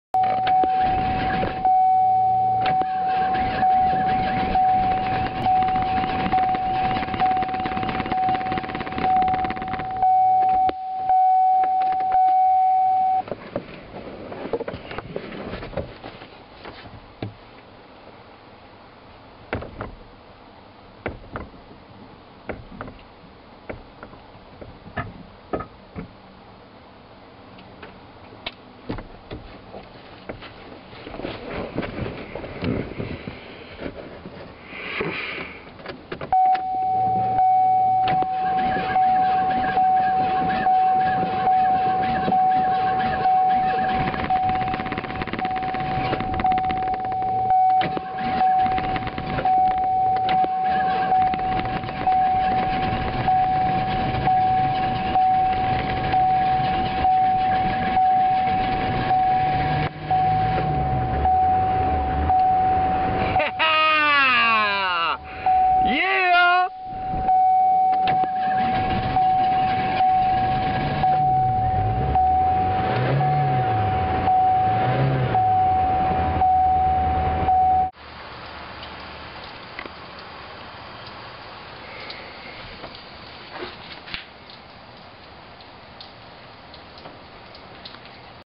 Starting the TurboCoupe
Getting my new 1988 TurboCoupe running